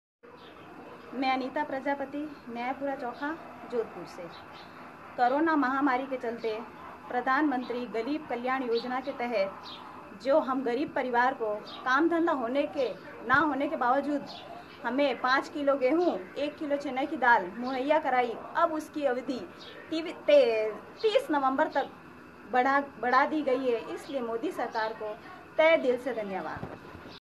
Anita Prajapati from Jodhpur is thankful to PM @Narendramodi and the PMGKAY (Pradhan Mantri Garib K
Anita Prajapati from Jodhpur is thankful to PM @Narendramodi and the PMGKAY (Pradhan Mantri Garib Kalyan Anna Yojna) for ensuring they do not have to worry about food for the next five months.

மித்ரன் பிரஸ் மீடியா அசோசியேஷன்
Mithran Press Media Association